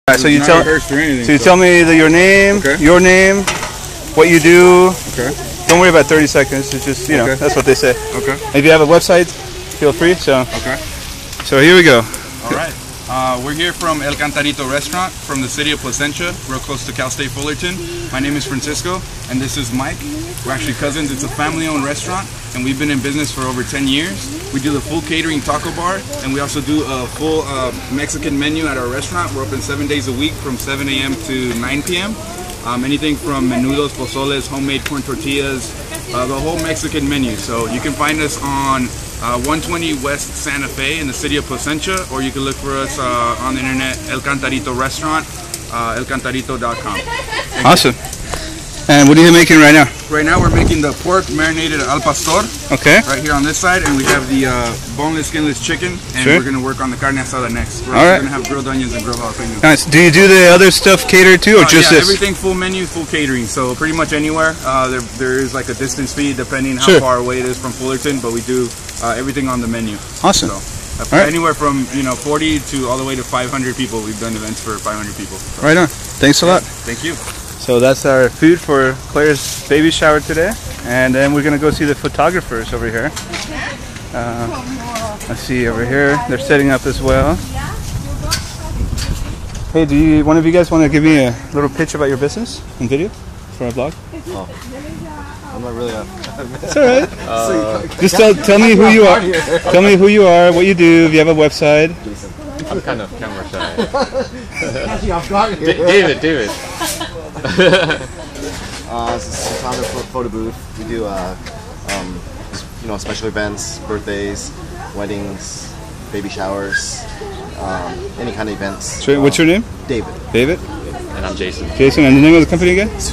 El Cantarito Restaurant from Placentia
El Cantarito came to cater Claire Gonzalez' baby shower and their food was awesome. Everyone liked them and I'd hire them in a heart beat for another event. Great value, great flavor and great execution.